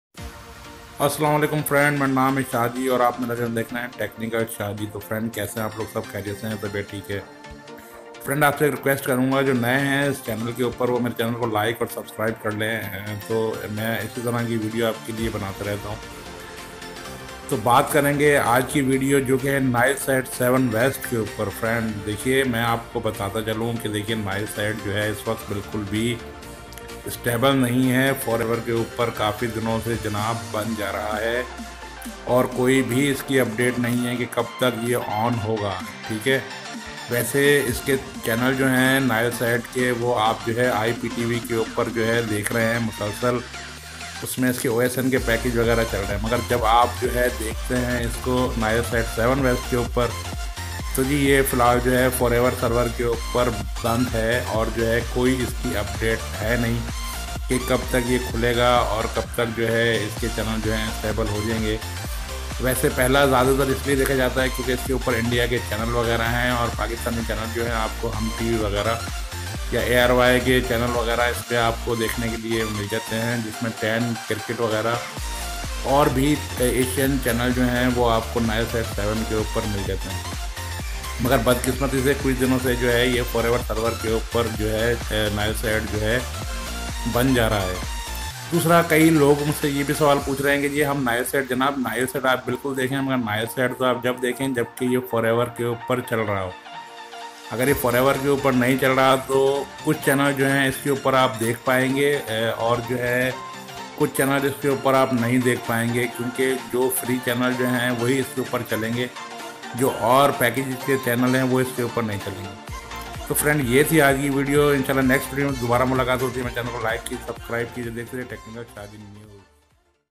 Nilesat7w Nilesat 7w New Update on Forever server !!
Hi Friends Nilesat 7 w ke jo pehle server pr chalta tha mgr ab nahi chal rha hai kisi bhi server uper nahi chal rha hai